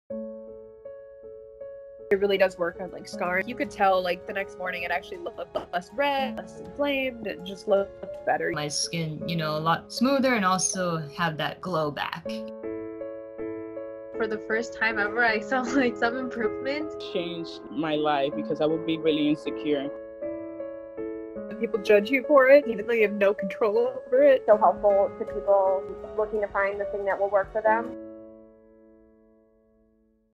More Than A Skincare Brand
Banish started from a personal cause. Through that discovery, Banish shares the confidence and positivity it brings to acne sufferers all over. It's more than a skincare brand.